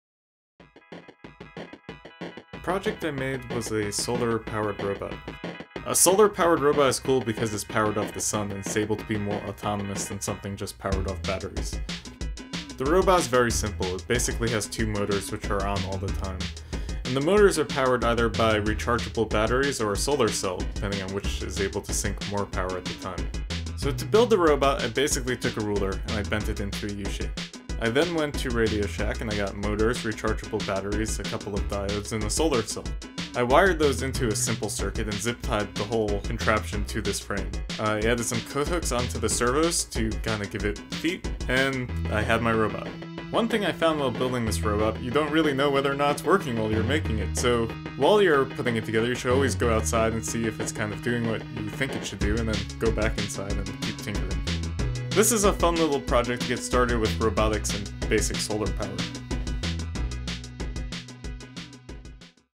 Solar-Powered Robot
A while back I made dozens of robots which were in large part inspired by BEAM Robotics. For those unfamiliar, BEAM is basically a special method of robot building with an emphasis on biology, electronics, aesthetics, and mechanics (hence the acronym BEAM). One thing that sets BEAM apart from other approaches to robotics is its insistence on using radiant energy (predominantly solar power) and its tendency towards reuse and minimalism. While I heavily borrowed from the BEAM ethos and aesthetics, the robots that I built were not quite the same (they were all battery powered for starters).

Since BEAM robotics was such a large source of inspiration, I always wanted to try my hand at building a solar robot. However, rather than simply building another BEAM robot, I decided to incorporate solar into my own style of robot building. Instead of having it be powered entirely off the sun, I decided to incorporate rechargeable batteries. This means that at any given time the motors can be running off either the batteries or solar panel, depending on which can provide the most power. The solar panel is also recharging the batteries when the sun hits it. This allows the bot to run off of the sun, but not be entirely reliant upon it to move.

I think my approach merges the two styles nicely, and is a fun and simple experiment in robot building.